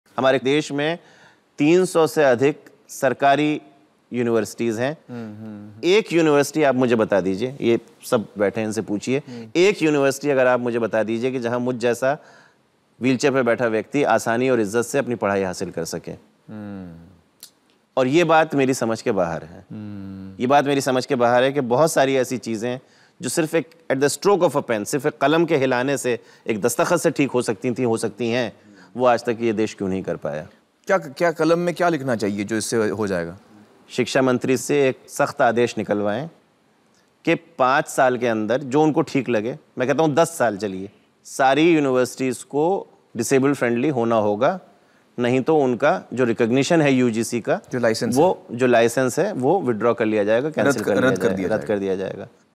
Persons with Disabilities - Education hurdle
Javed Abidi says one big issue is that universities in India are not barrier-free -- not even government ones.